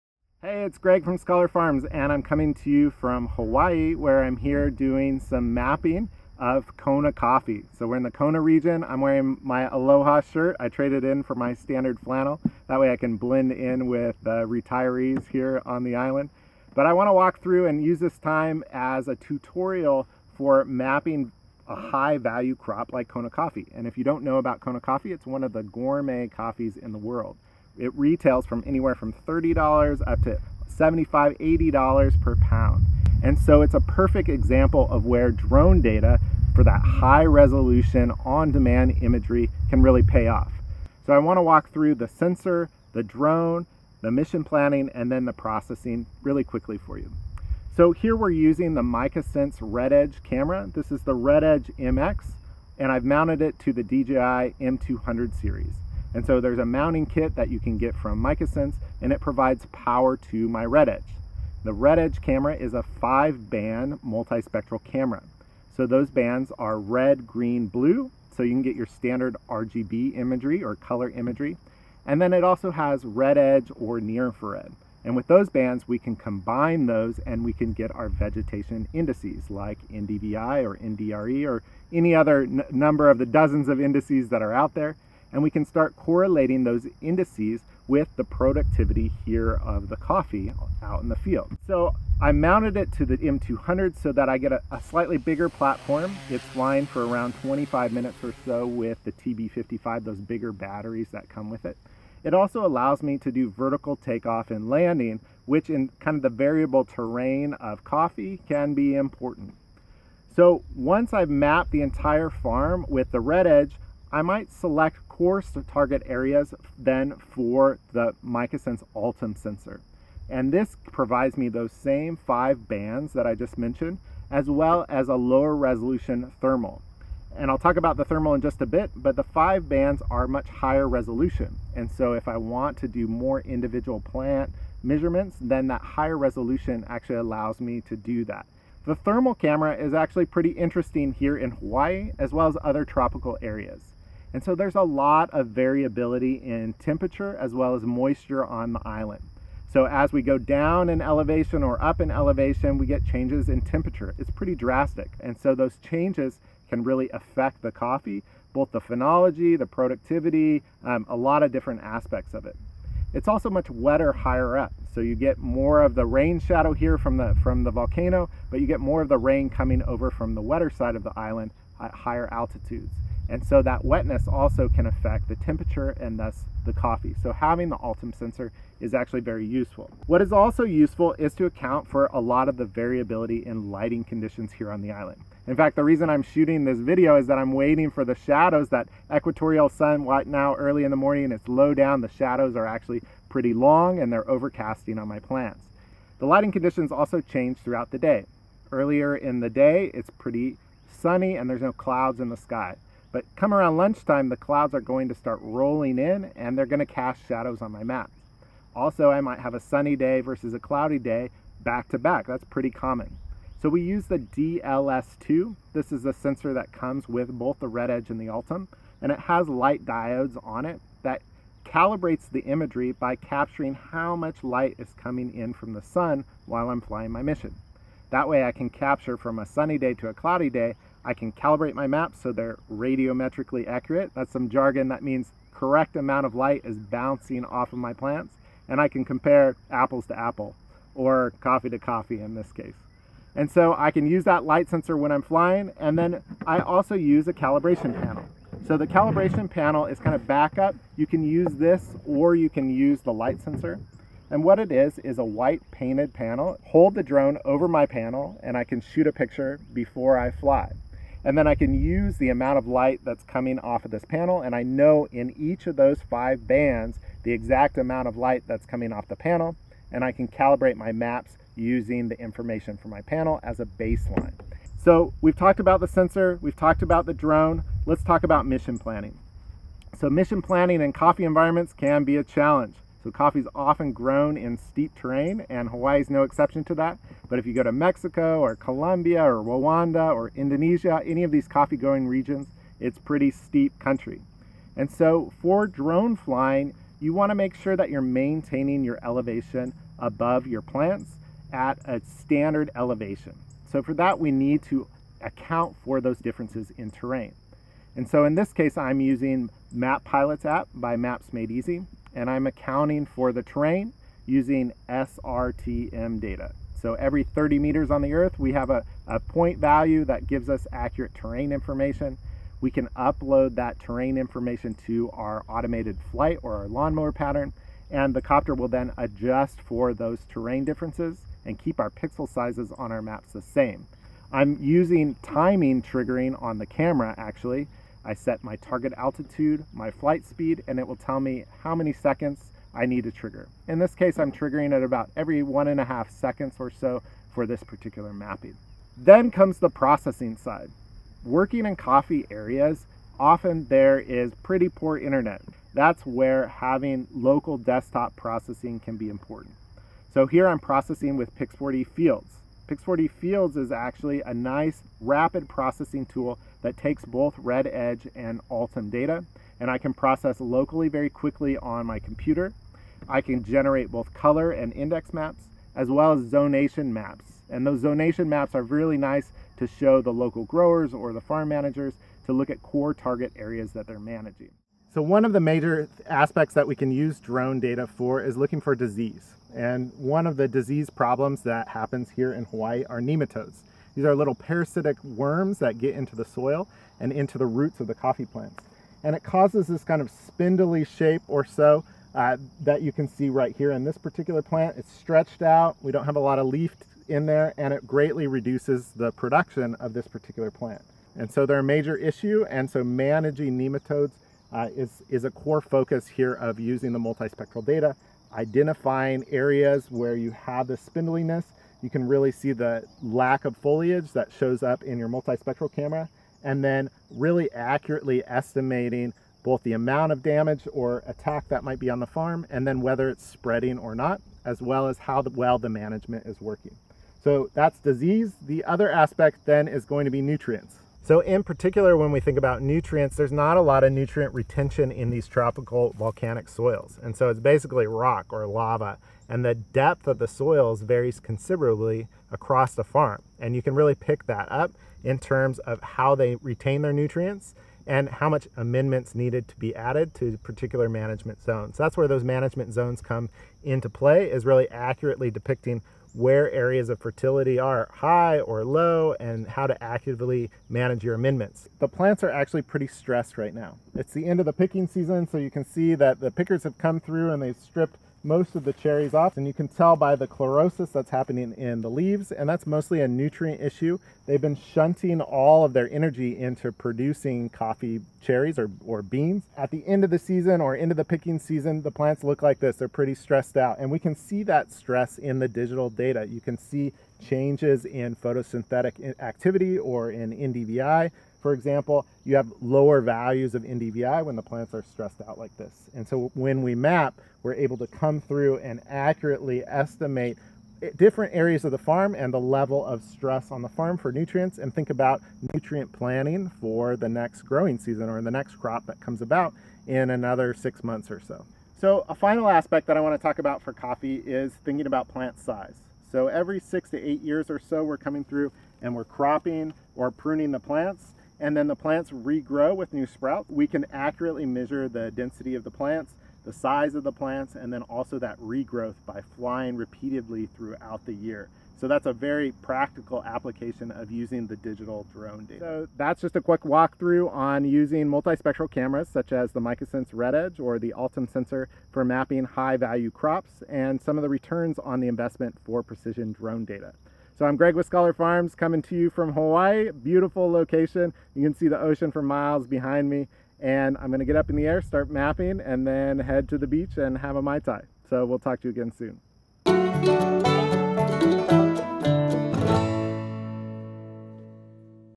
How to Map High-Value Crops with Drones
Dr. Gregory Crutsinger from Scholar Farms using the Rededge MX and Altum multispectral cameras from Micasense in Seattle, Washington for mapping of Kona coffee in Hawaii, , a high value crop that can retail from $30-80 per pound..  These narrow band cameras are purpose built for vegetation mapping in precision agriculture.  These sensors were mounted to the DJI M200 series, but the Rededge MX can also be mounted to the Sensefly eBee, Delair UX11, or the Wingtra 1.  The imagery is then processed in a photogrammetry software.  Pix4DFields is used as a rapid field processing tool for generating vegetation indices, such as NDVI or NDRE.  Terrain following features were used in Map Pilot by Maps Made Easy for adjusting altitude and maintaining pixel size.